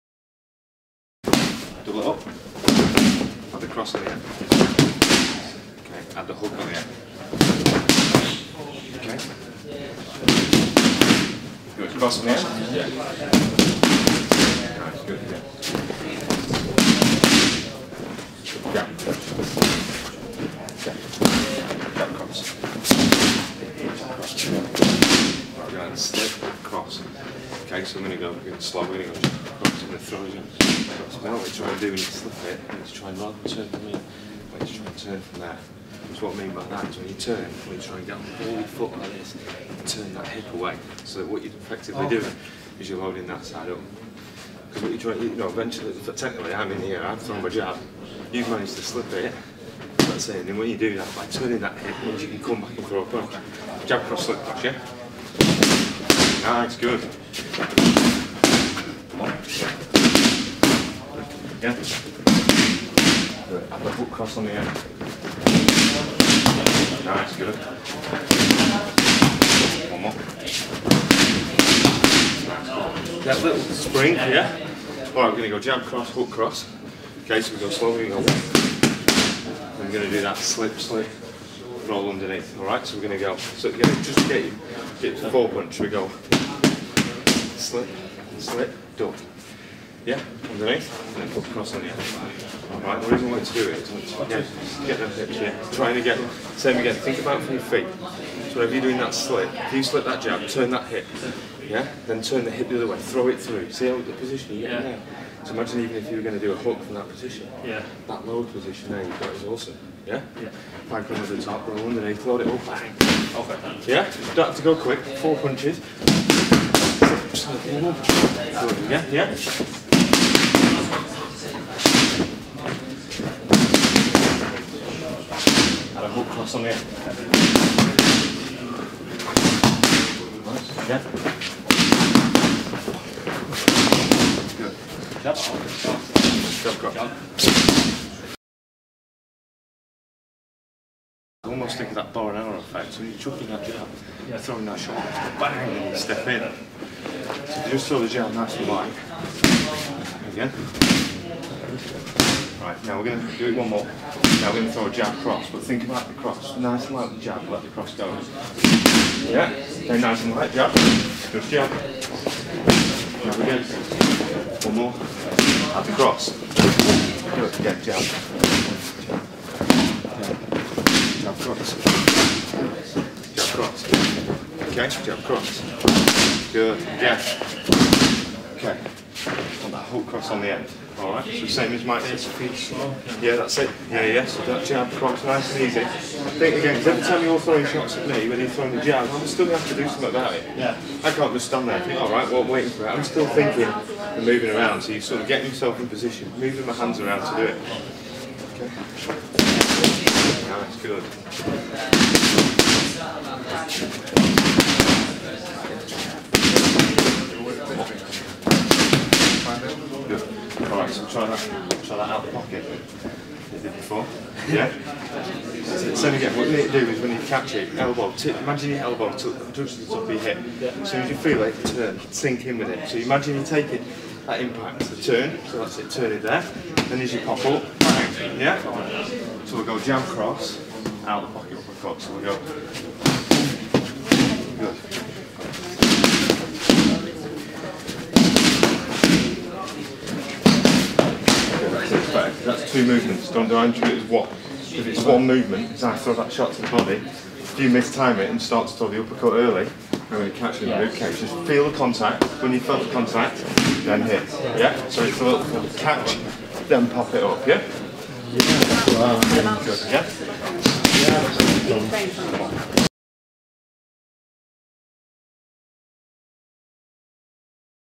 Hatton Academy's Jon Eade takes MMA star Paul Sass on the pads
Jon Eade, Hatton Academy's training and educational director, shows UFC star Paul Sass the basics of boxing.